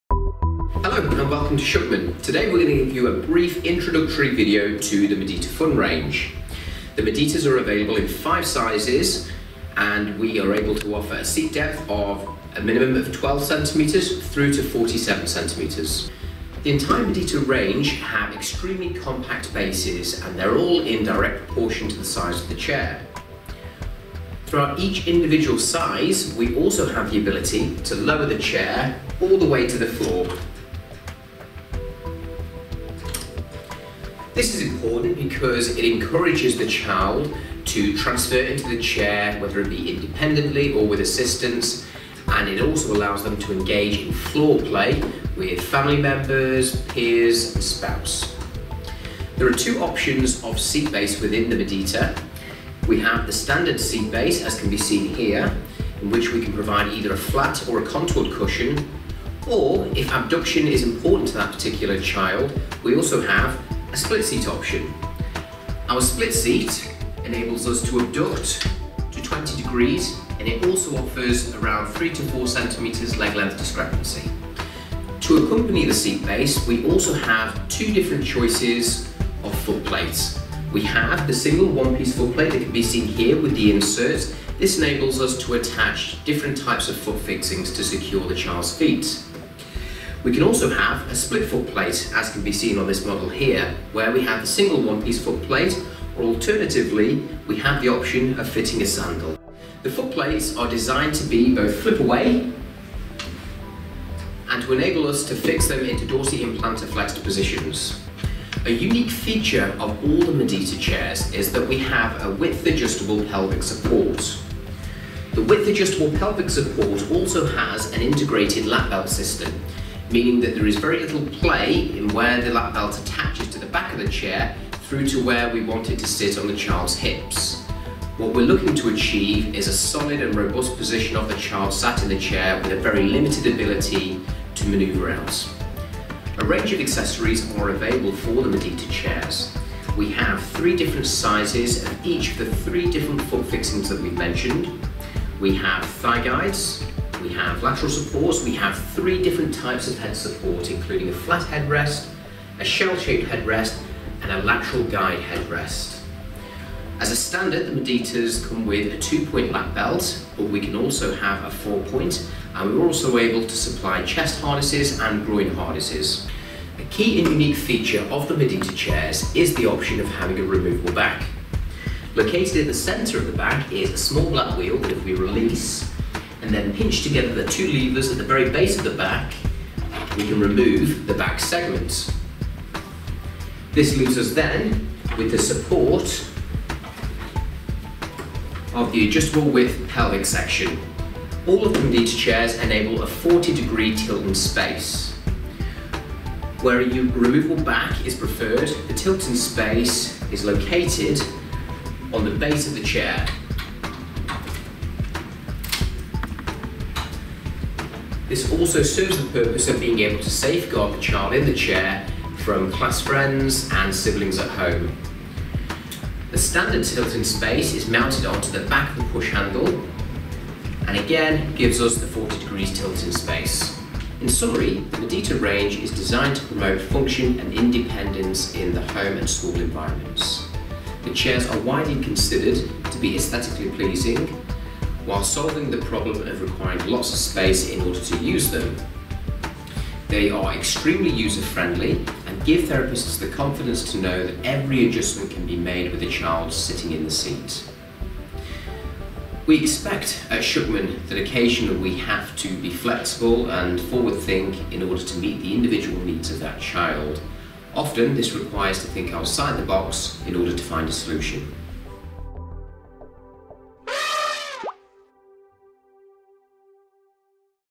schuchmann: madita fun (English)
madita-fun.I am your integrative therapy chair.Whether at the breakfast table or in the play corner; I will put you on a level with your friends. I will move you up and down. It is easy due to the support from my gas pressure springs. Use my lever to tip me forwards and backwards. I can be adapted to your body and your tastes: I am available in lots of great colours. Did you know that my backrest can be removed? This trains your back musculature and helps you to sit up straight.